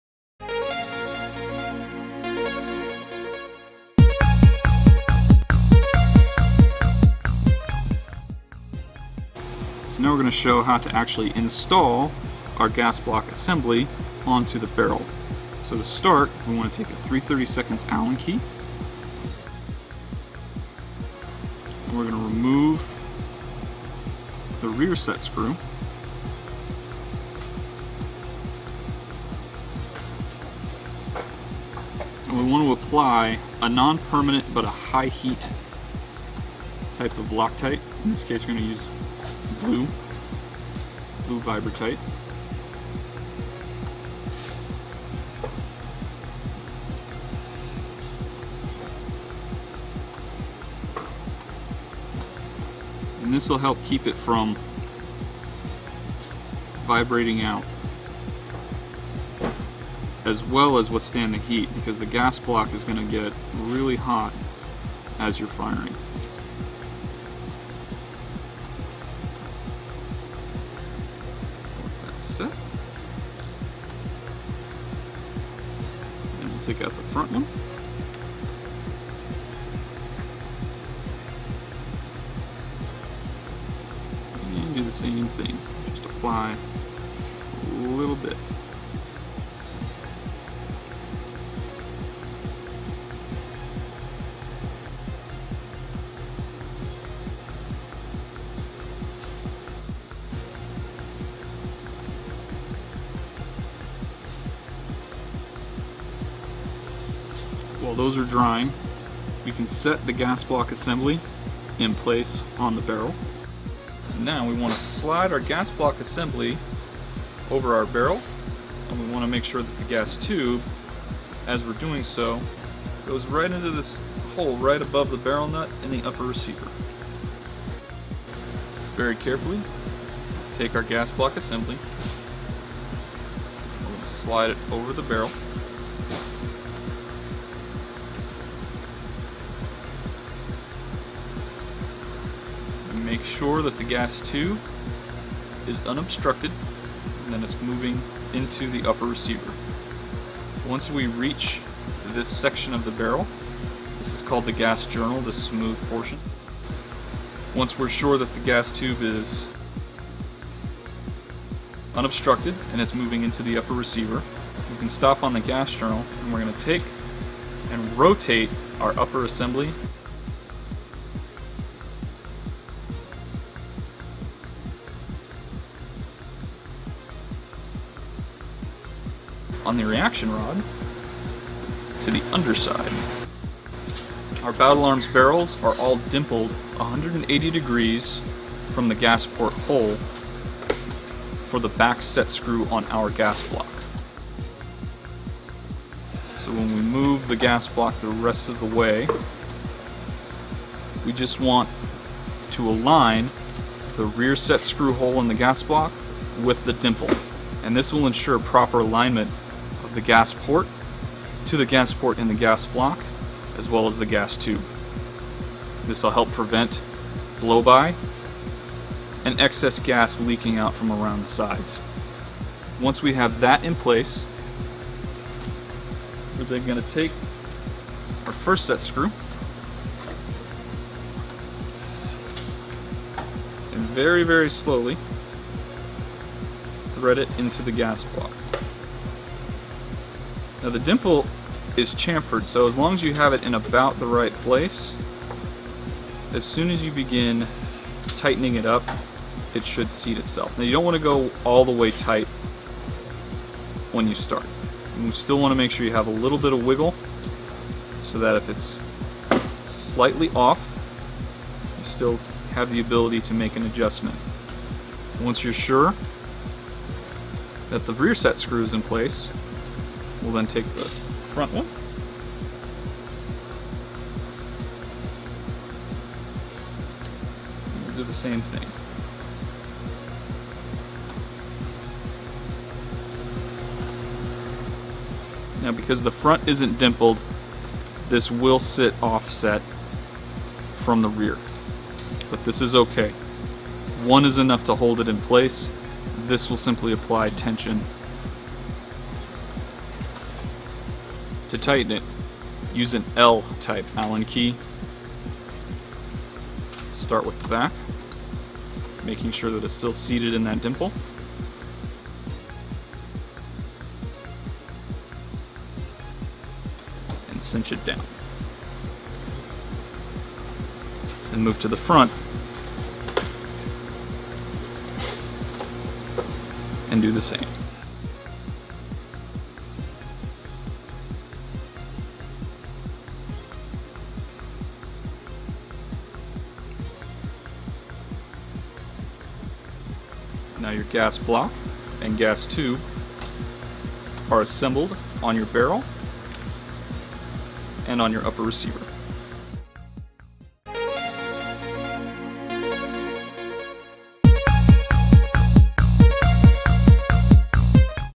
THE UPPER Gas Block Assembly on the Barrel Part 5
With the gas block and gas tube pinned together, we can now install them onto the barrel and upper receiver assembly.